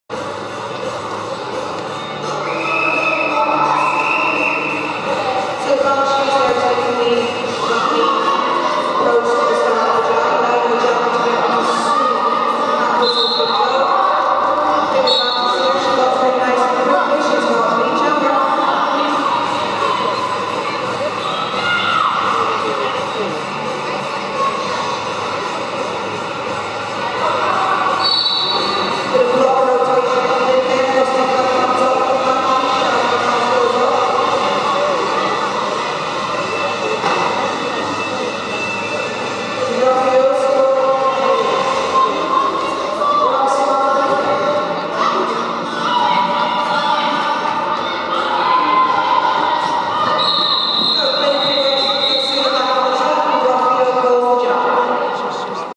Slaughtership Down: GCRG Intraleague. Jam 10 Period 1
Slaughtership Down: Granite City Roller Girls Intraleague (with guest skaters from the rest of Scotland's Roller Derby Leagues)

12 Feb 2012 @ Bell's Sport Centre, Perth

This is the 10th Jam of the 1st Period. This is a power jam, for Bigwig's Banshees, with Mallory Powers (9) jamming.